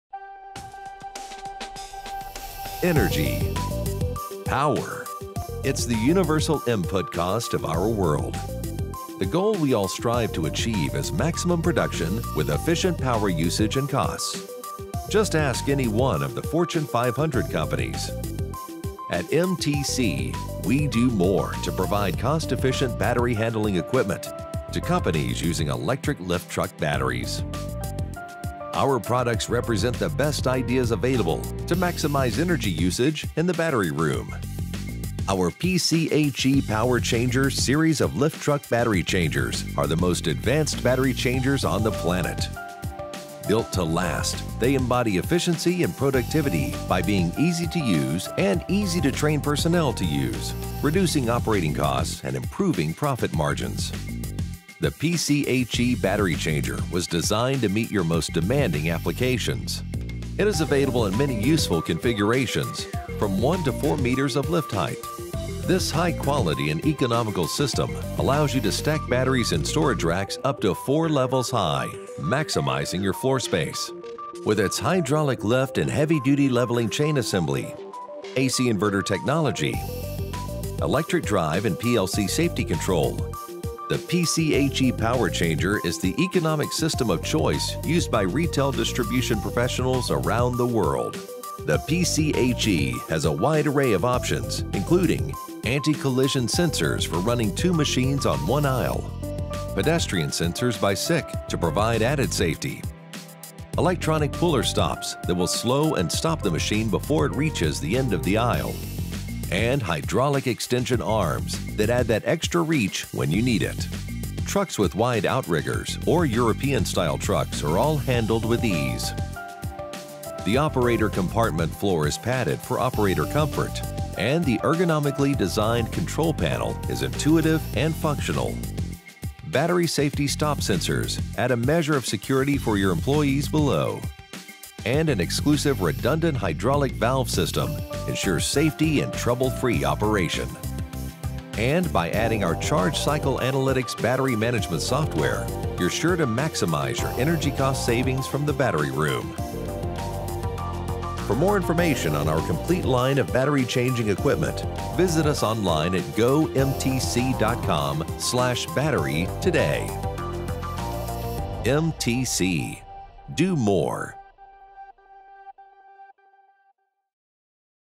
MTC Power Changers PCHE
MTC's innovation in high volume battery changers sets us apart from the competition. The MTC PCHE contains no hydraulic lines on the operator platform. It uses a delayed platform raising action allowing operators to make quicker changes and increase productivity.